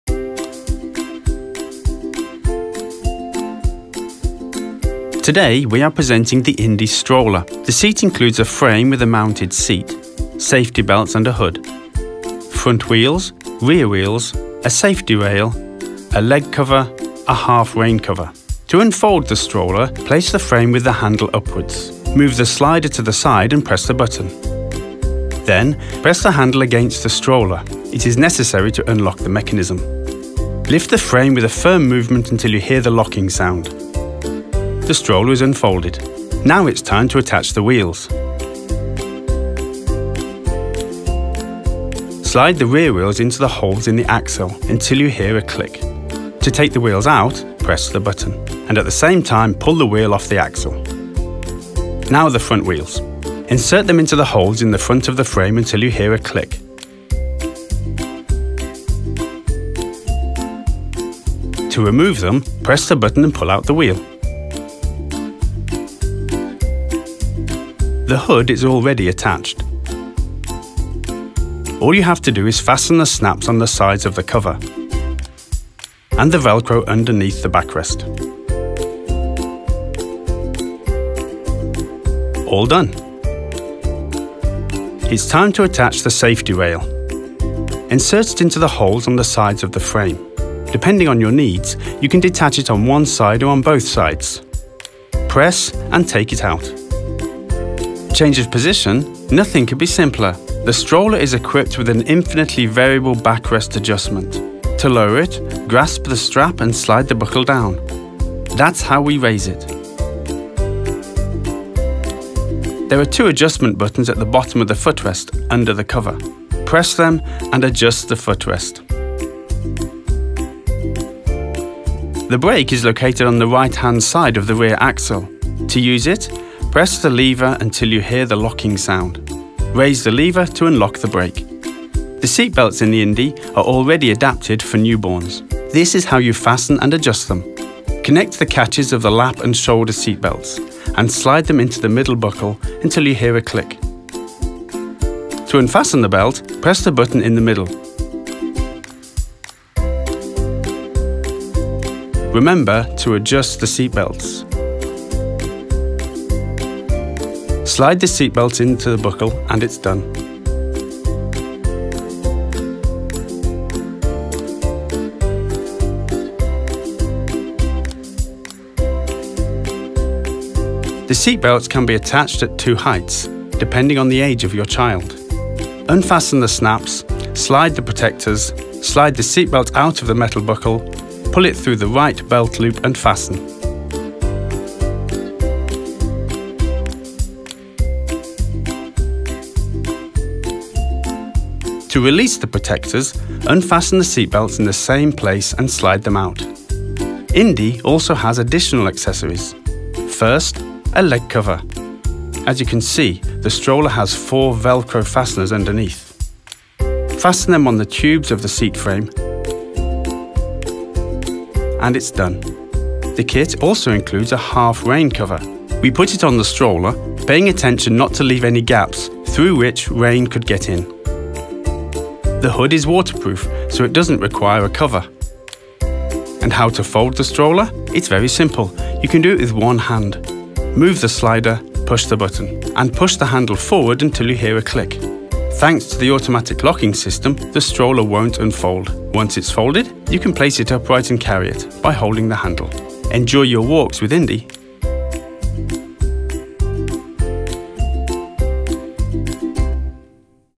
Kinderkraft INDY stroller How to video | Tutorial
Learn all about the INDY stroller by Kinderkraft – a city stroller that's easy to manoeuvre. The video will show you how to release the automatic lock, unfold the frame and correctly clip in the wheels. We'll explain simply how to affix the material on the back of the hood and how to use the pushchair. We'll also demonstrate how to lock the wheels to drive straight and how to fold the INDY stroller in just a few seconds. The pushchair features a guard rail that can be removed from both sides – this makes it easier to put your child in and take them out of the stroller. You'll also learn how to correctly remove the guard rail to avoid damage, and how to properly adjust the backrest. Parents often don't know how to operate the STOP & RIDE brake – the video will also demonstrate how to release it correctly. Learn how to fold and unfold the INDY stroller and enjoy the practical solutions on your walks.